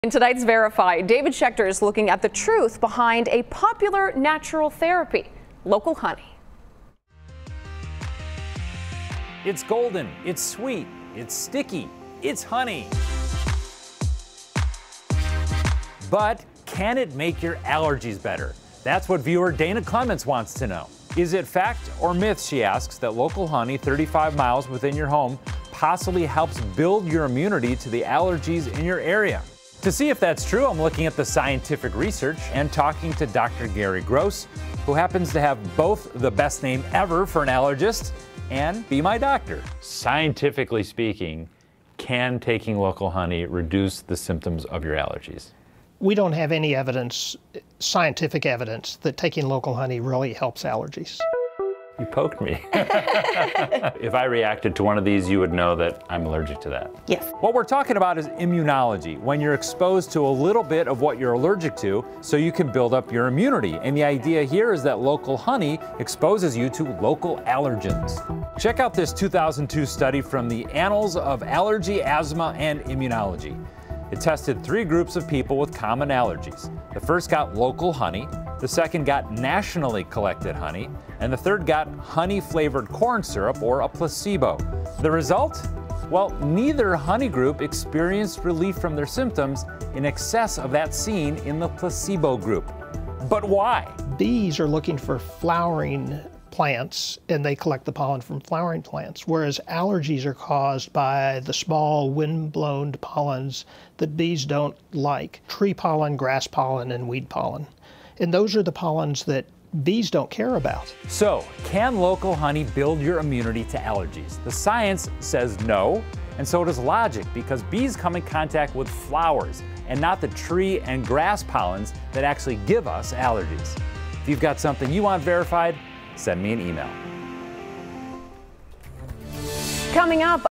VERIFY: Think local honey is good for allergies? Here's why you're wrong.
What does the science have to say?